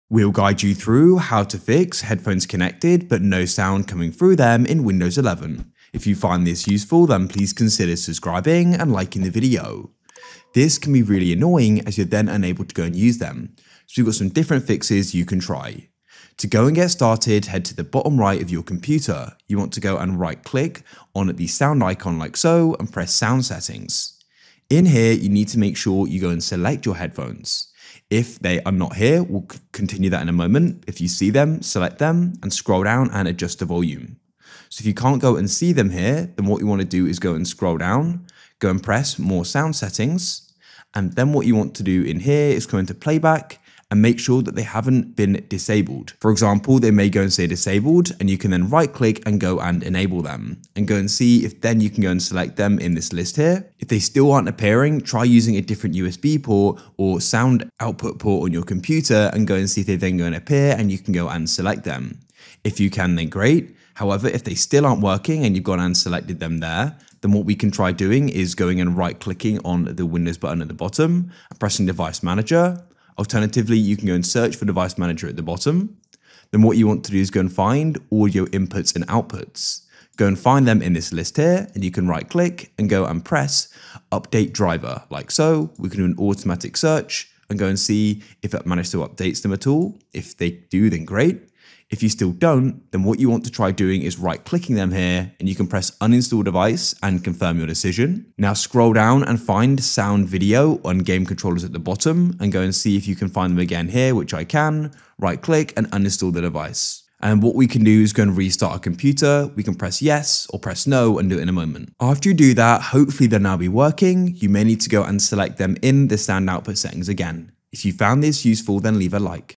How To Fix No Sound From Headphones On Windows 11 - Full Guide
Learn how to fix no sound from headphones on windows 11 in this video.

GuideRealm